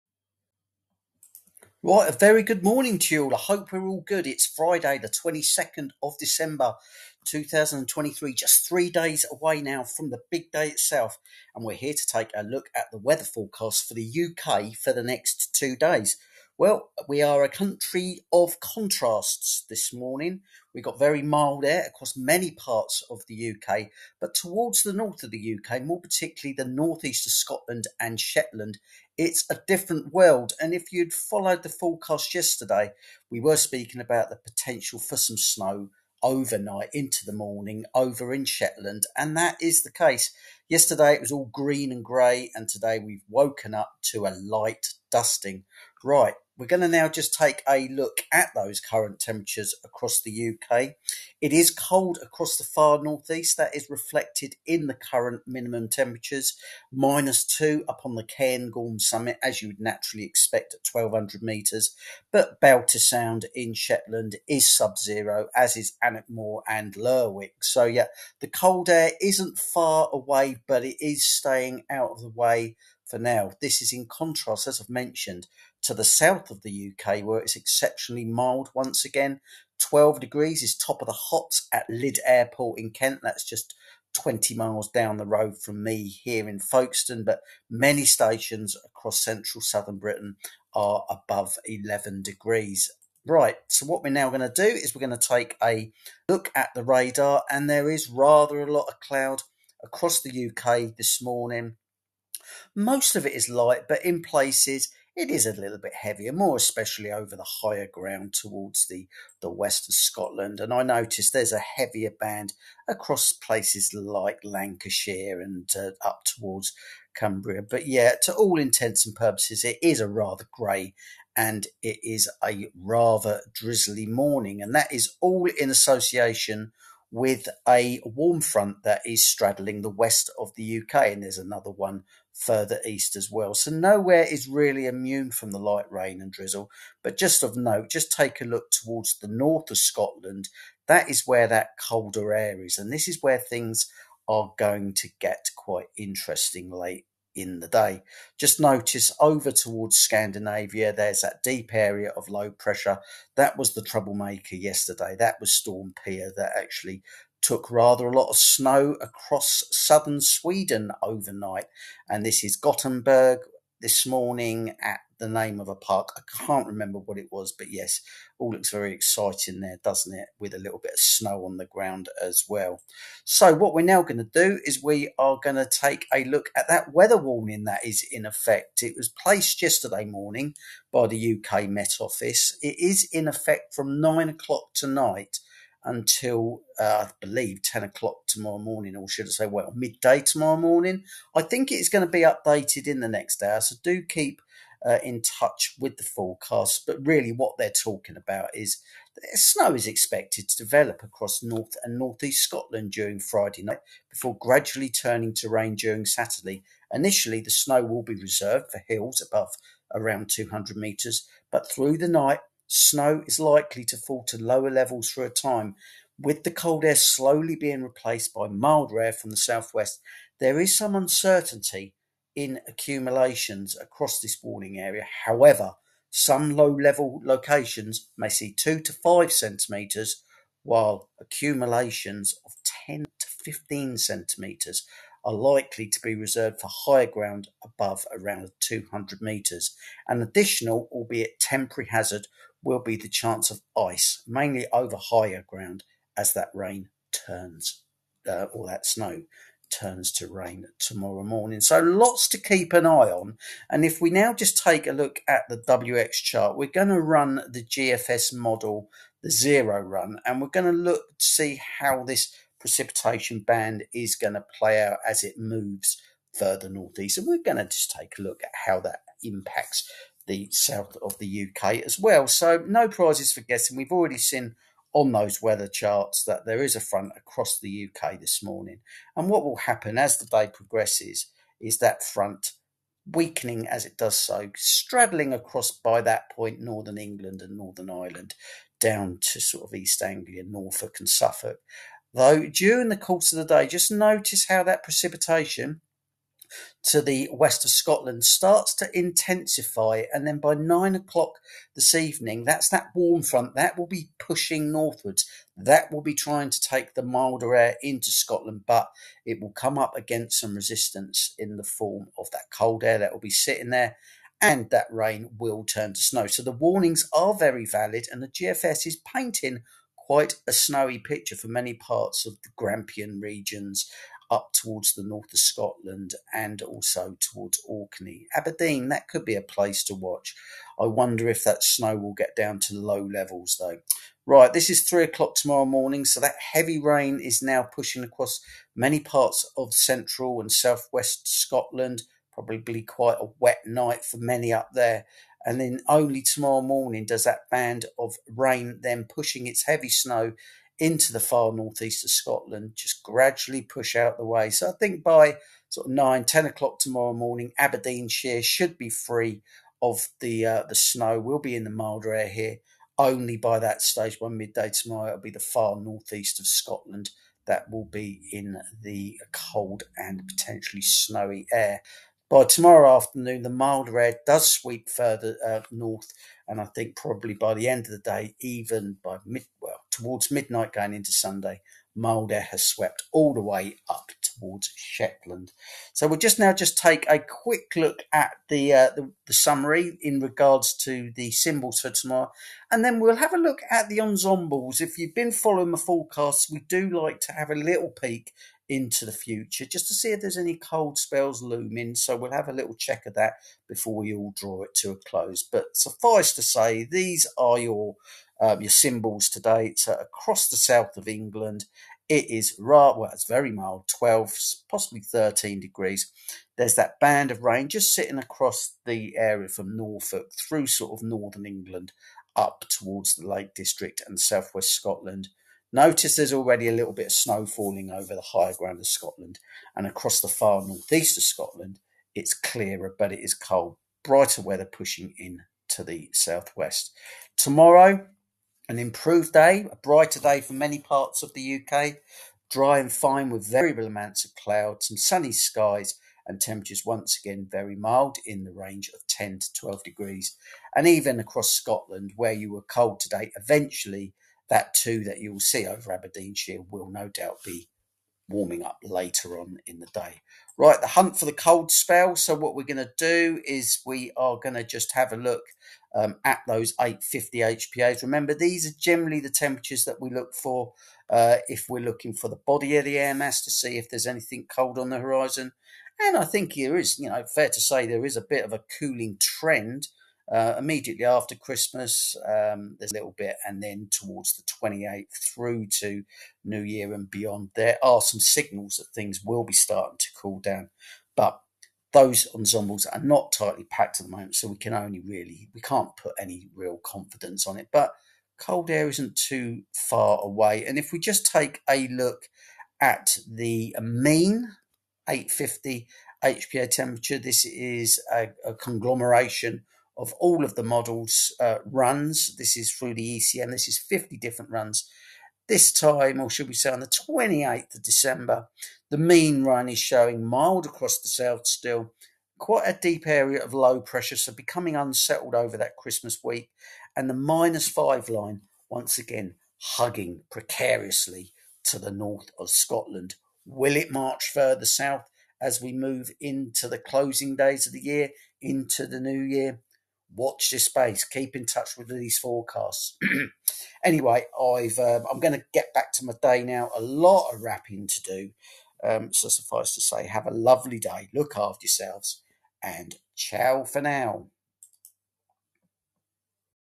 A NATION OF CONTRASTS - UK WEATHER FORECAST - 22/23RD DECEMBER 2023.
The UK is a nation of contrasts. Cold air in the far NE brings snow warnings with it. A warm front moving from the south creates the battleground as Springlike warmth affects the south.